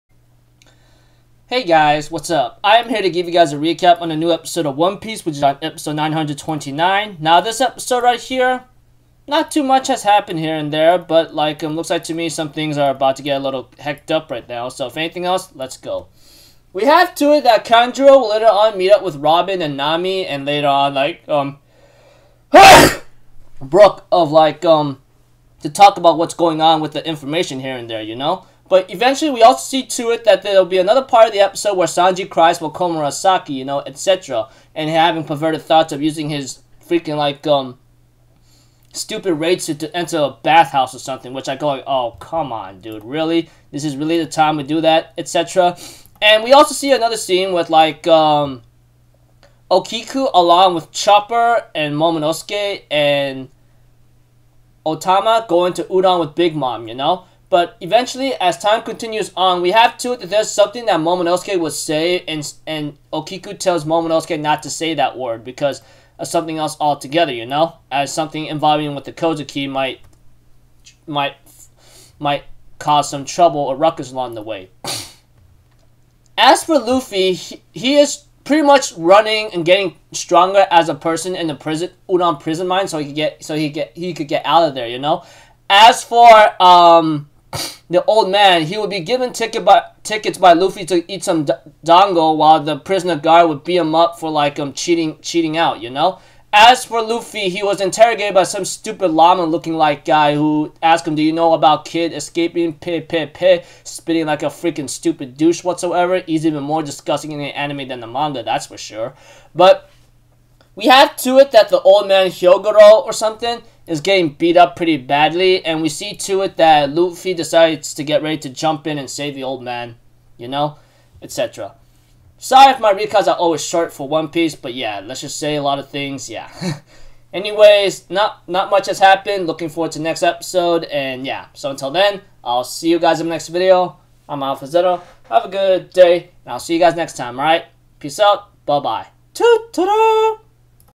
One Piece ワンピース Episode 929 Recap.
Well Luffy is going to cause some chaos in the mines. That is for sure.